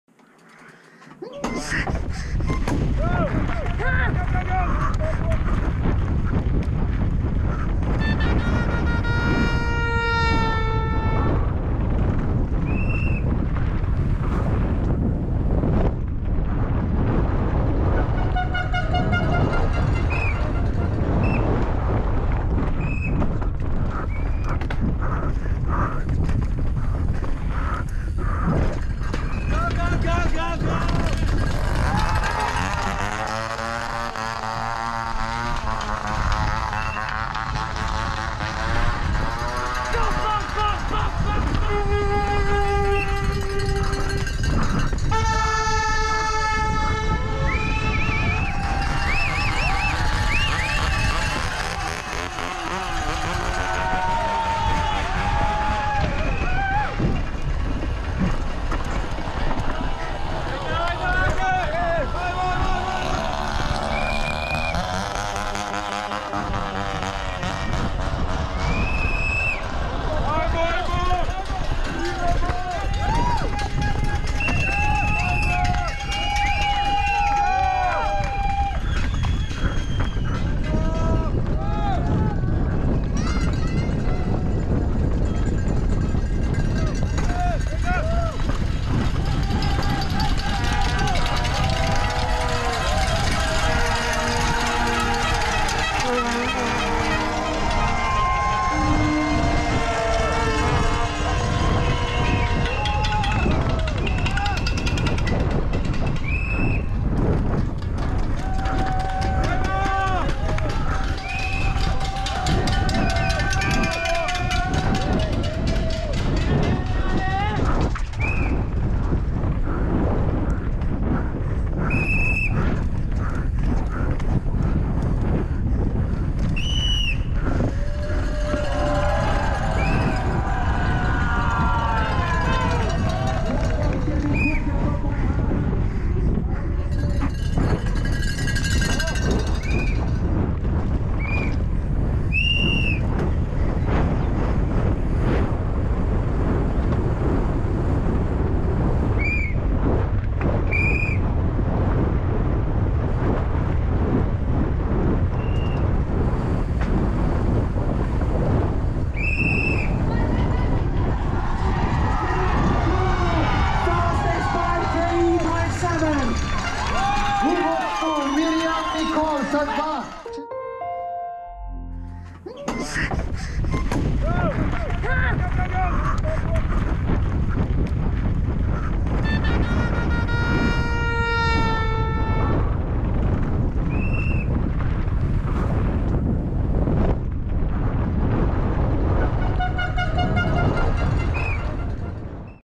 UCI MTB 2018: Myriam Nicole's charged POV winning run.
►Click to watch the first DH race of UCI MTB World Cup in Croatia Myriam Nicole (FRA) sprinted hot out of the gate and charged down a ruthless track in Losinj with nothing held back, resulting in a hard-earned first place at the first stop of the 2018 UCI Mountain Bike World Cup. Second place goes to Rachel Atherton (GBR) and third to Tahnee Seagrave (GBR).